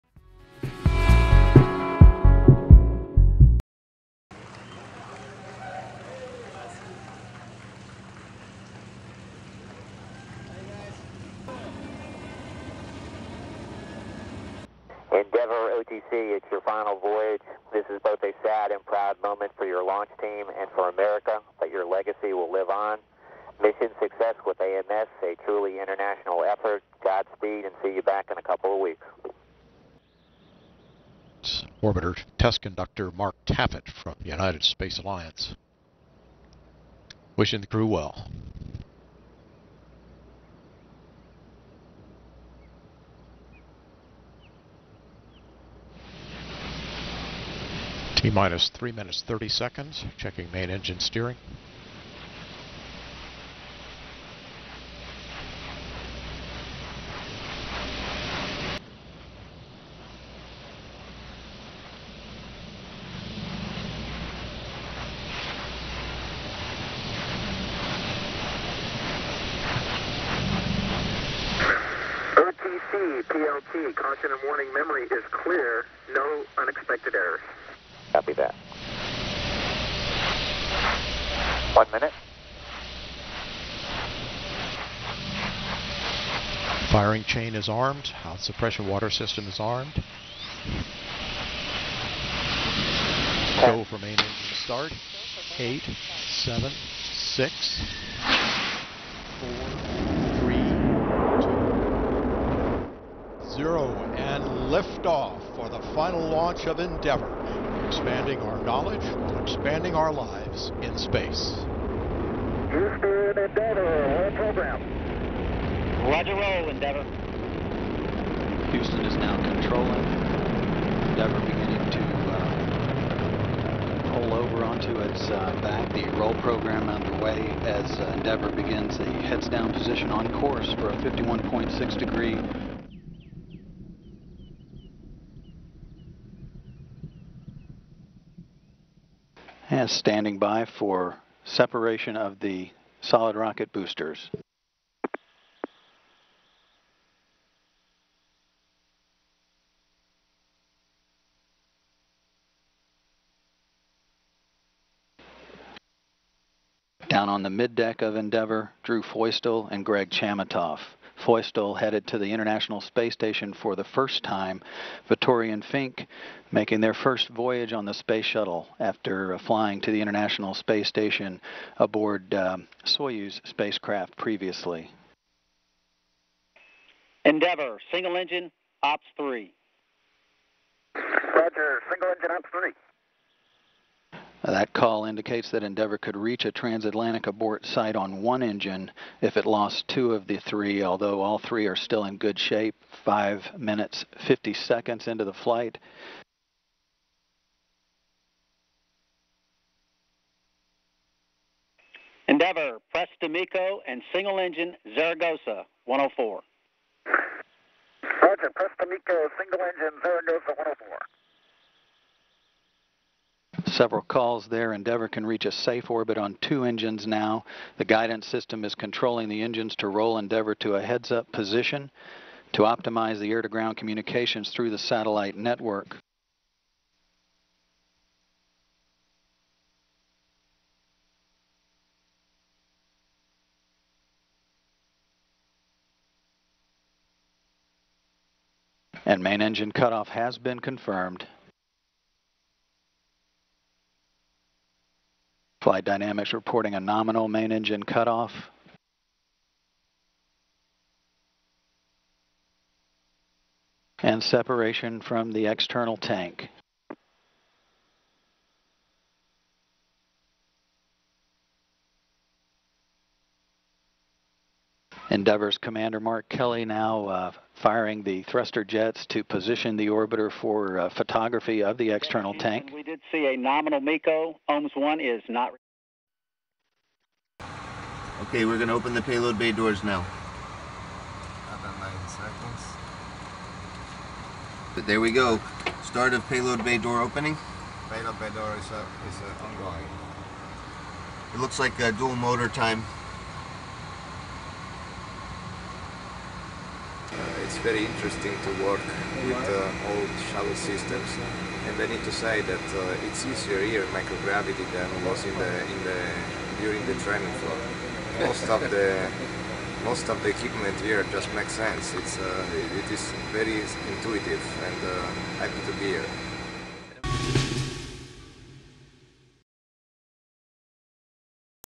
STS-134 / DAMA mission
Space Shuttle "Endeavour" lifted off on her last mission to the International Space Station. The STS-134 mission was launched at 12:56 GMT (14:56 CEST) on 16 May 2011.
Roberto's mission is named DAMA in reference to the search for the mysterious dark matter that will be conducted by the 6.9-tonne fundamental physics payload, the AMS-02 Alpha Magnetic Spectrometer, probably the most ambitious science payload ever launched to the Station.